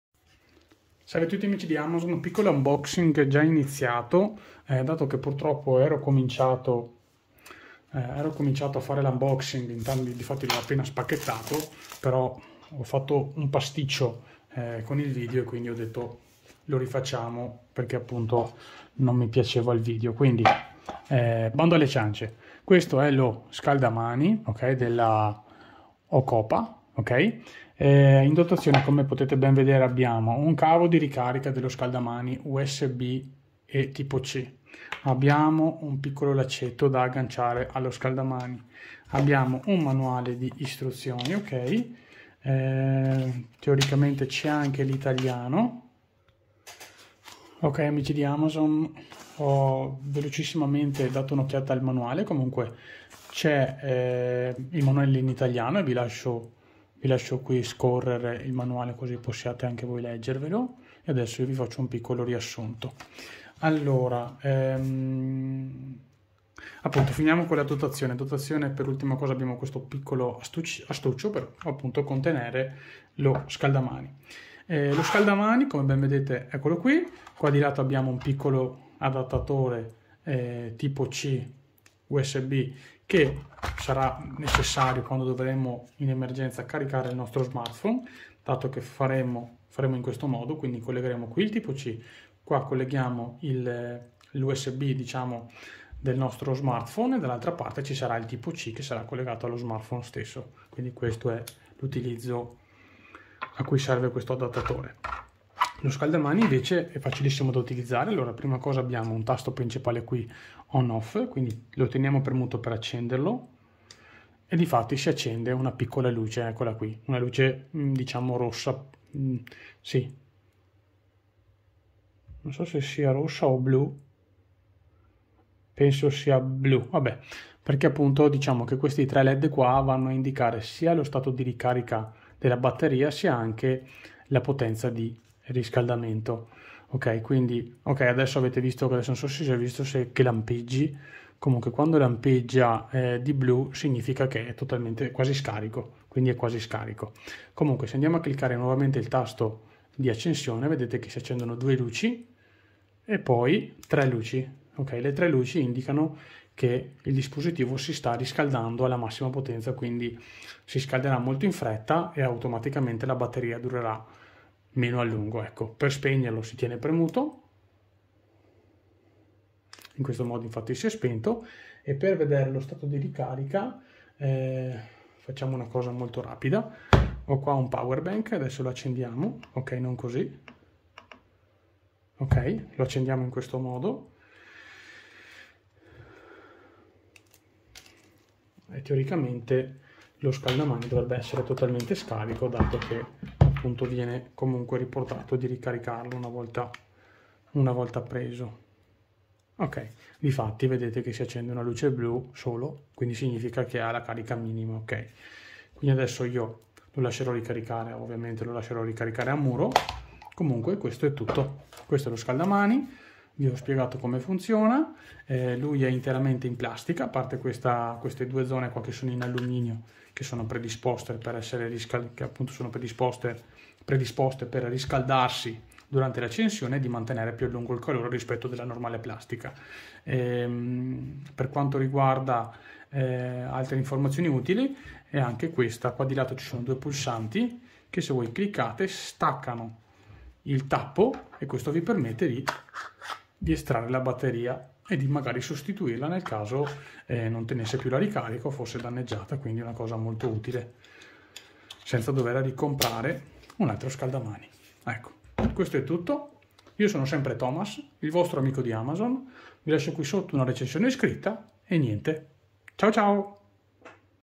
OCOOPA Ricaricabile Scaldamani, Prestazioni e autonomia eccellenti, fattura un pò meno!
► Per ulteriori informazioni, visitare il sito